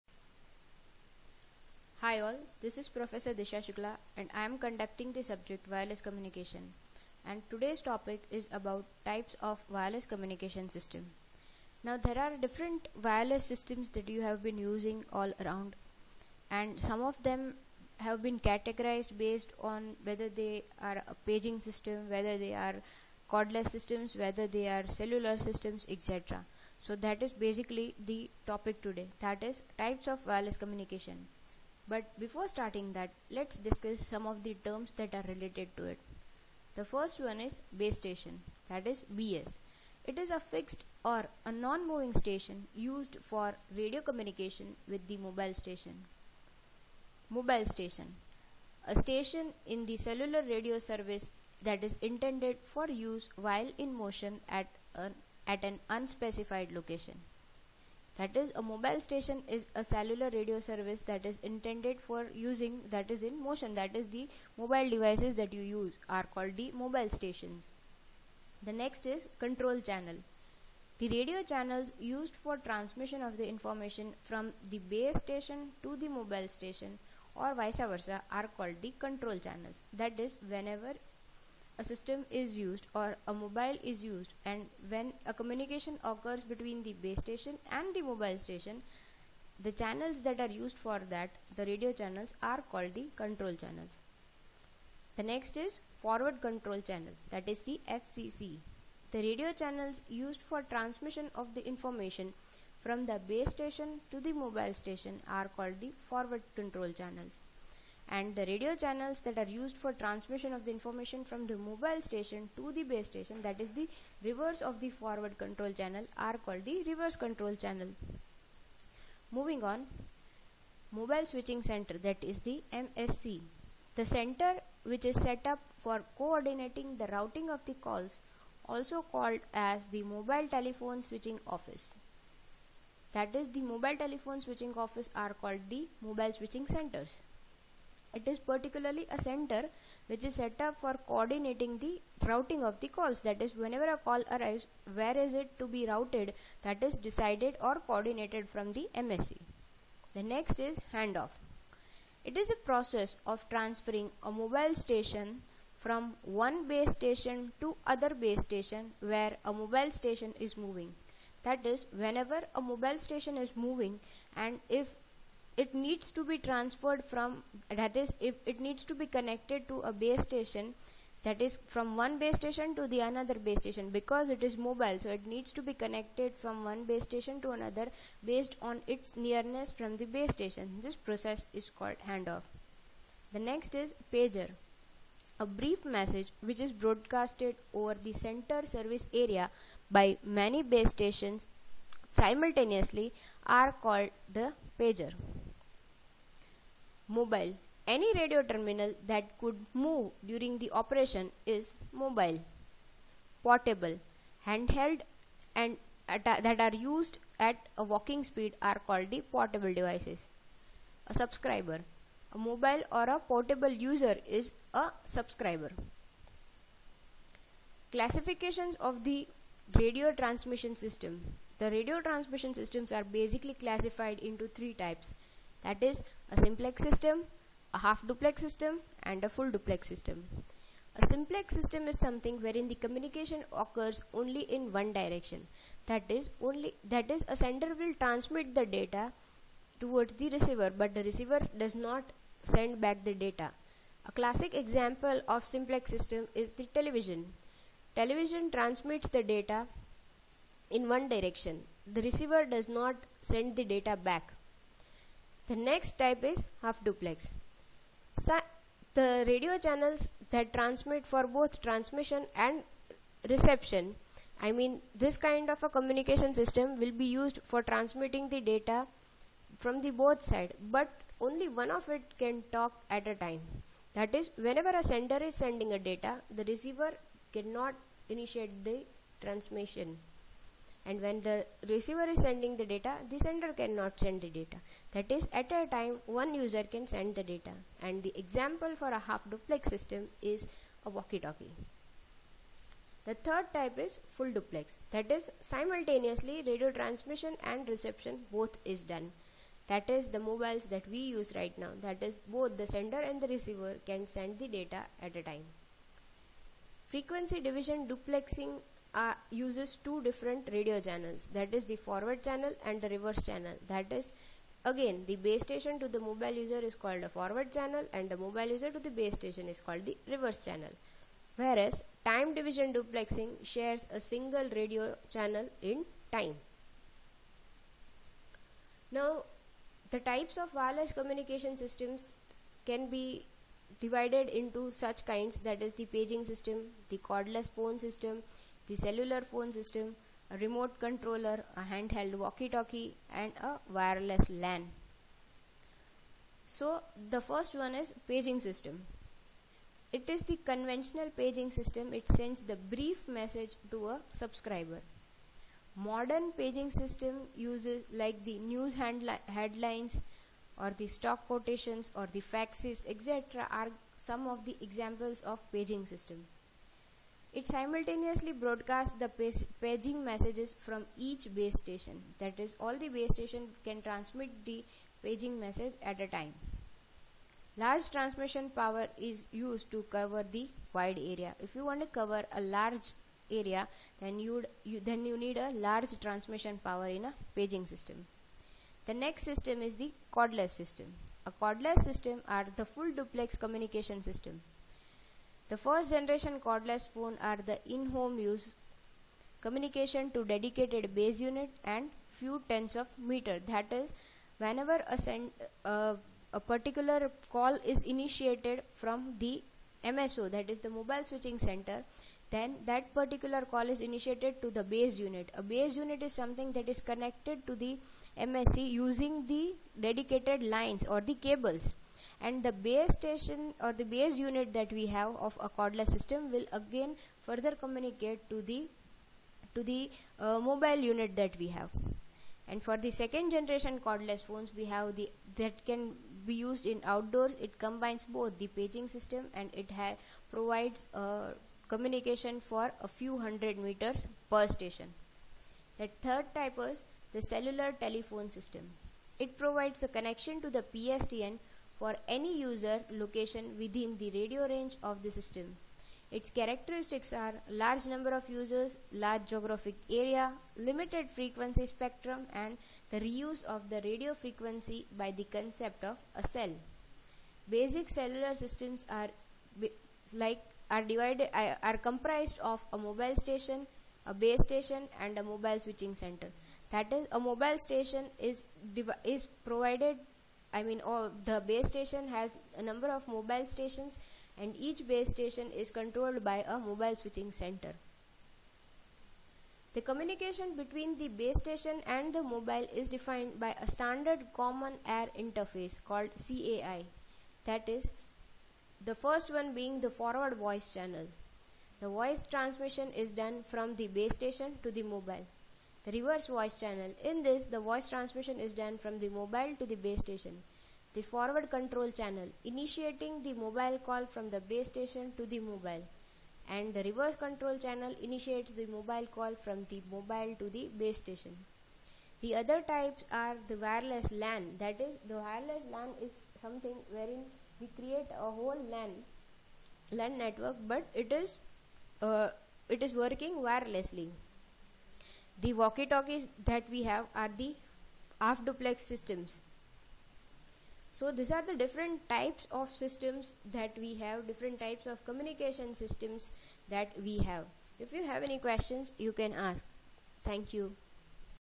Types of Wireless Communication
The video discusses about the various types of Communication system around the world.